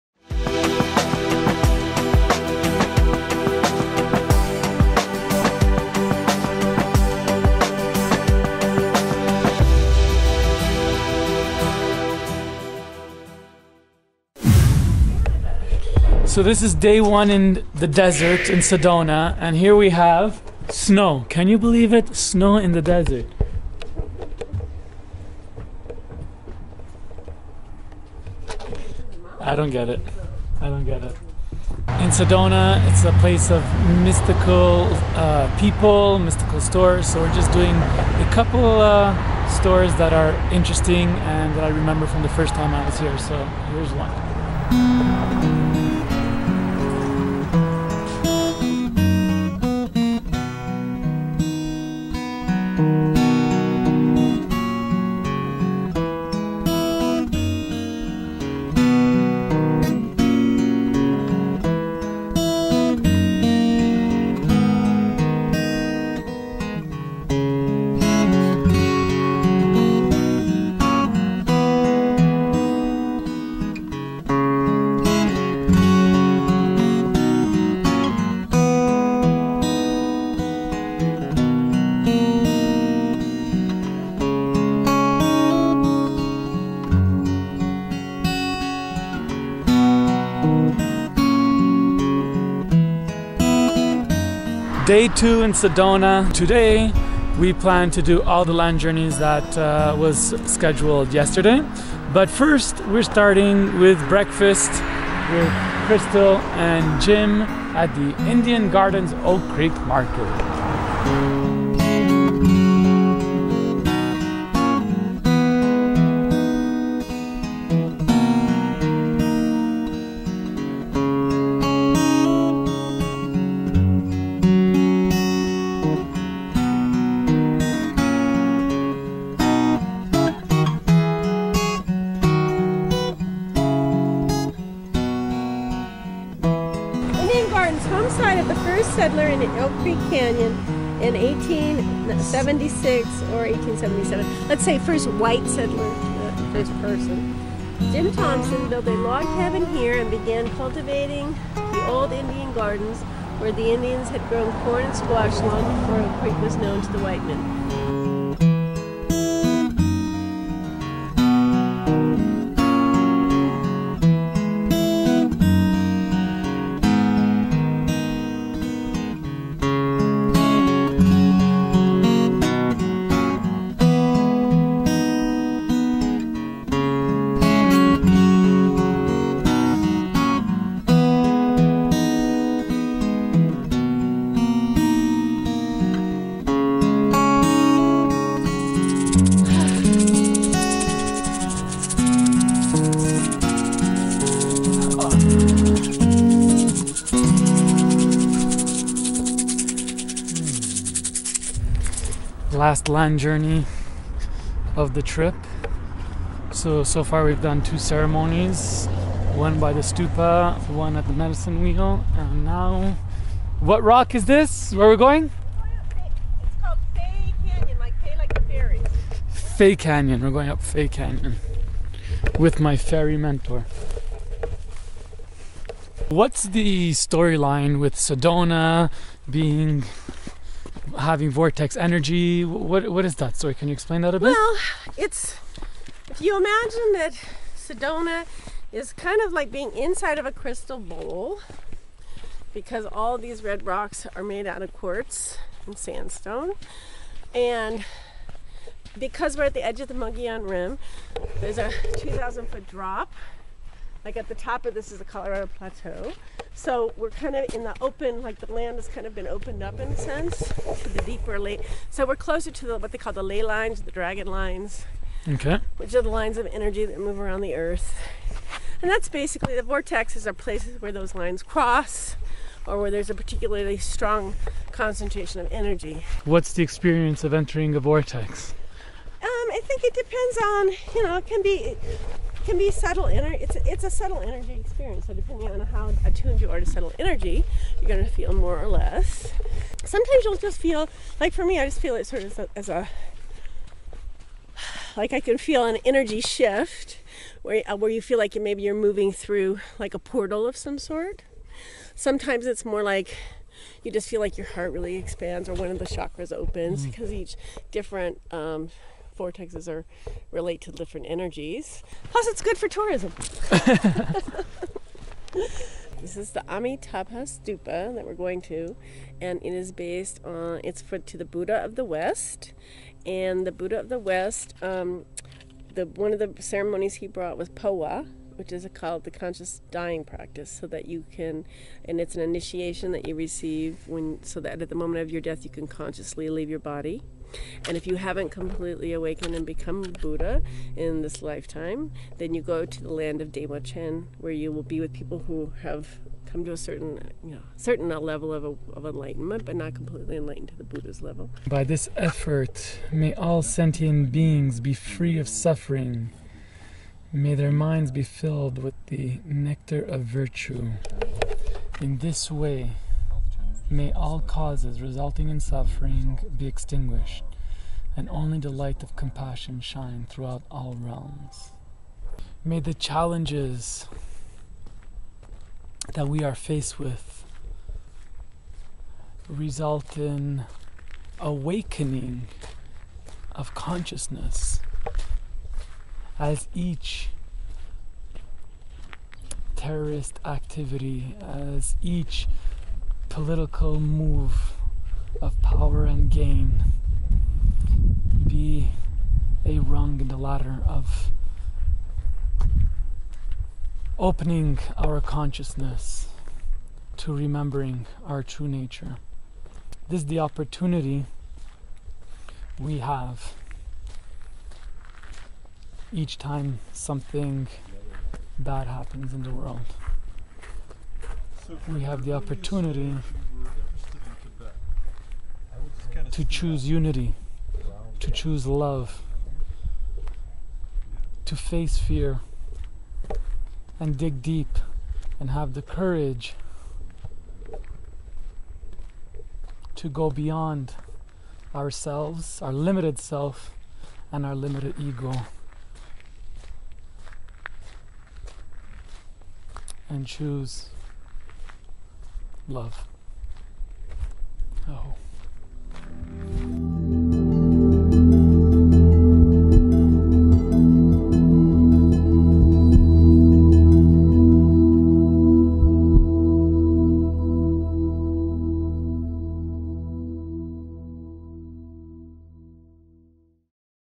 The Spiritual Playboy - Episode 51: On the land in Sedona
Welcome to The Spiritual Playboy Vlogs, following our guide Frank Mondeose in his worldwide adventures in spirituality and ecstacy!

In this episode - Accompanied by Crystal Dawn Morris, Frank visits the spiritual sights and sounds of Sedona, sharing some thoughts about darkness and light...

Edited by:
IN-NO-V8